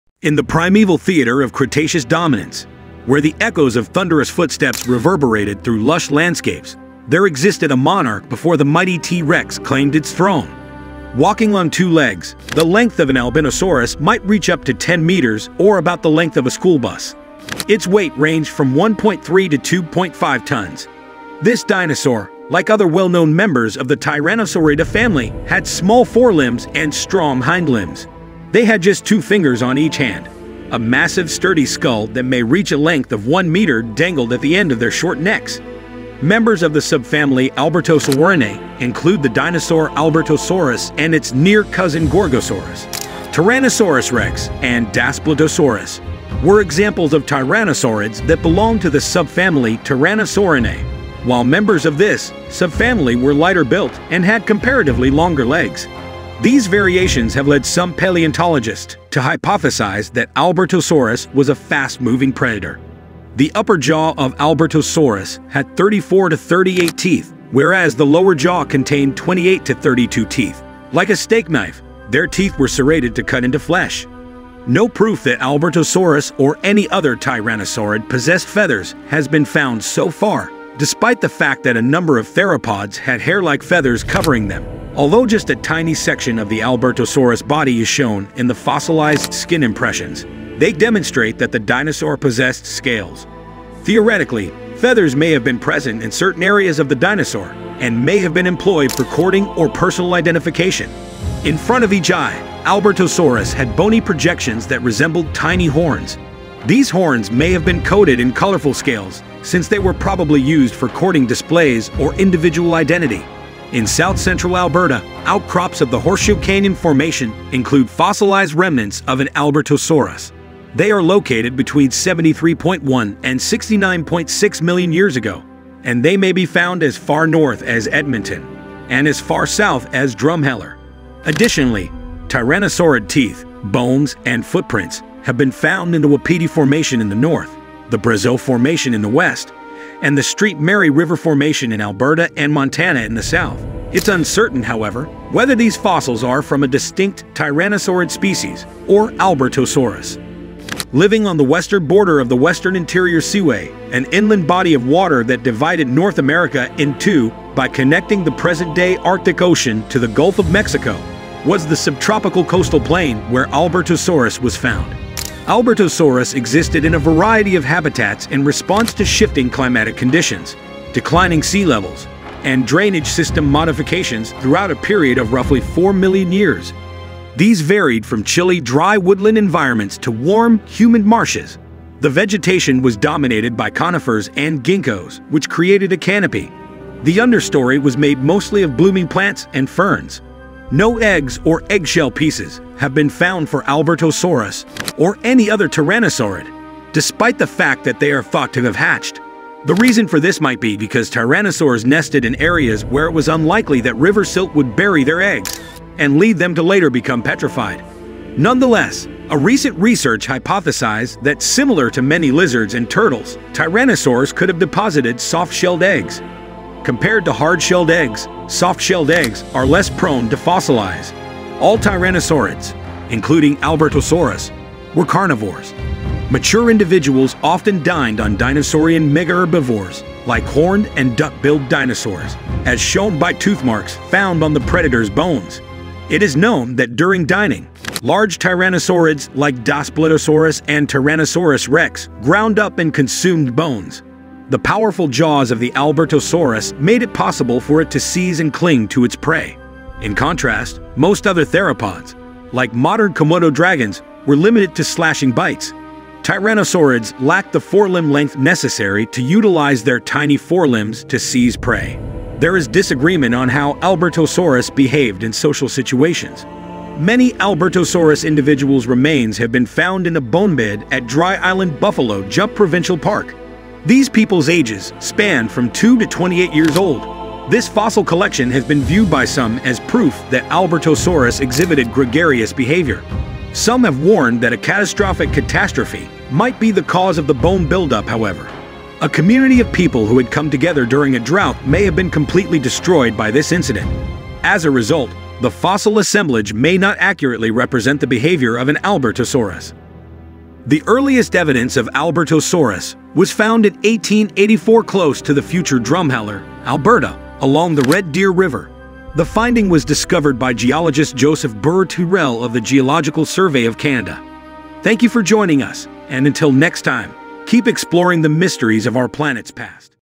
Albertosaurus | The Tyrannosaur That Was King Before T. Rex
Before the iconic T. Rex claimed its throne, there was another formidable ruler – the Albertosaurus! Join us on a prehistoric journey as we uncover the fascinating story of the Albertosaurus, a tyrannosaur that once dominated the ancient landscapes.

Did you know that Albertosaurus was a close relative of the famous T. Rex? Discover the similarities and differences between these two incredible predators, and learn how the Albertosaurus paved the way for its larger successor.

Immerse yourself in the prehistoric era and witness the thrilling moments when Albertosaurus roamed the Earth. From its hunting strategies to interactions with other dinosaurs, this video offers a glimpse into the life of this ancient king.